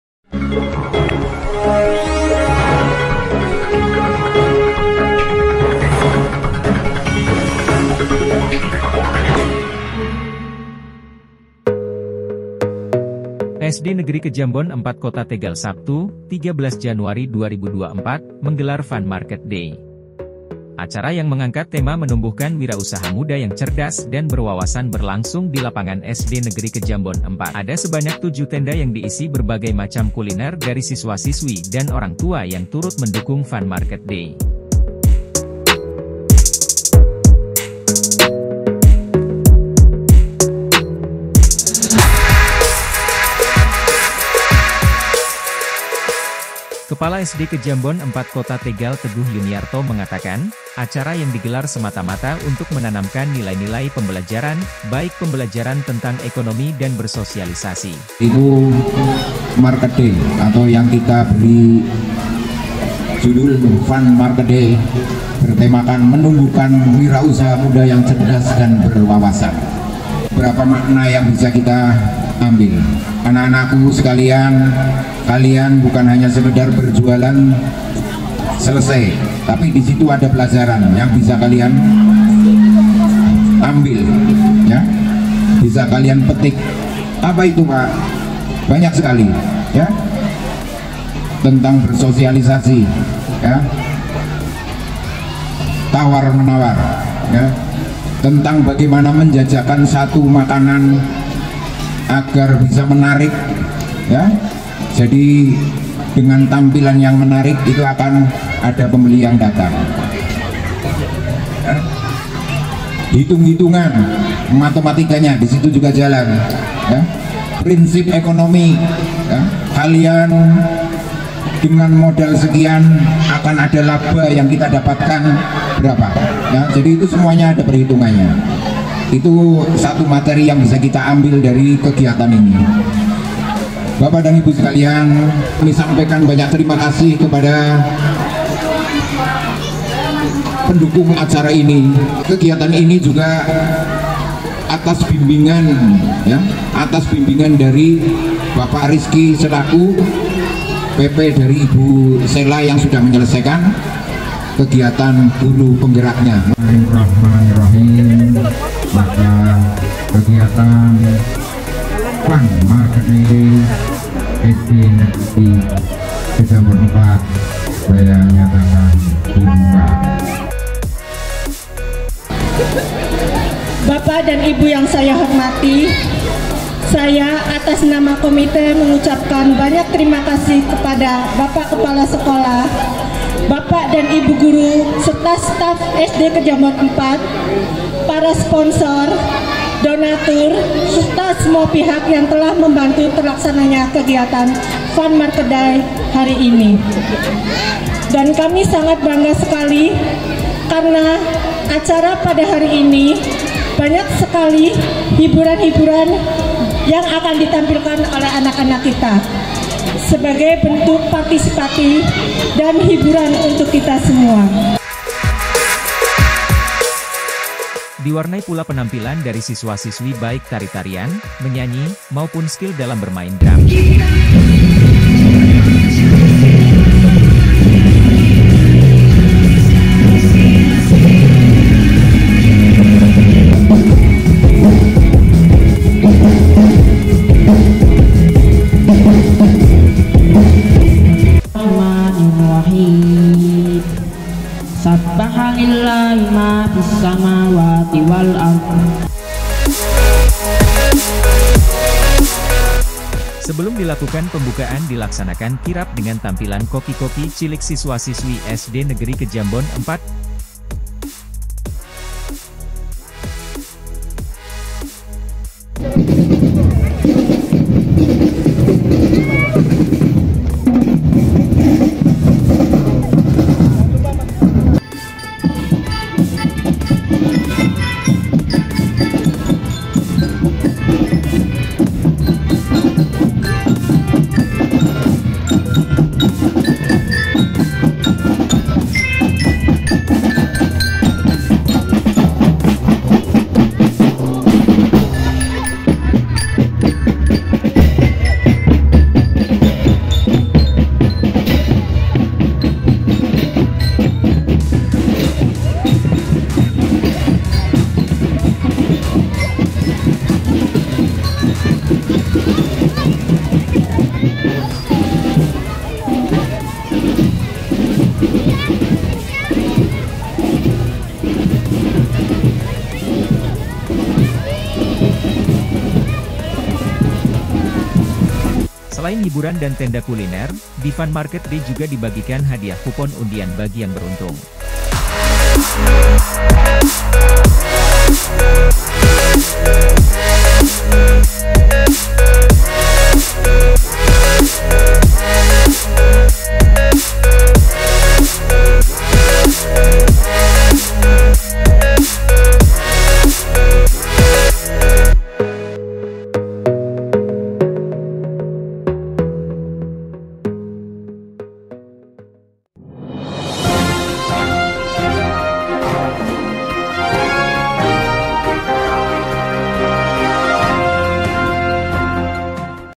Fun Market Day SD Kejambon 4 Kota Tegal Digelar Meriah, Tampilkan Berbagai Kuliner dan Bakat Siswa
Meriah! SD Kejambon 4 Kota Tegal Gelar Fun Market Day

SD Negeri Kejambon 4 Kota Tegal Sabtu (13/1/2024) menggelar Fun Market Day. Acara yang mengangkat tema Menumbuhkan Wirausaha Muda yang Cerdas dan Berwawasan berlangsung di lapangan SD Negeri Kejambon 4.

Ada sebanyak 7 tenda yang diisi berbagai macam kuliner dari siswa-siswi dan orang tua yang turut mendukung Fun Market Day.

Kepala SD Kejambon 4 Kota Tegal Teguh Yuniarto mengatakan, acara yang digelar semata-mata untuk menanamkan nilai-nilai pembelajaran, baik pembelajaran tentang ekonomi Deng bersosialisasi, pembelajaran tentang berwirausaha dan berkreasi membuat kuliner, hingga pembelajaran hitung-hitungan nilai transaksi.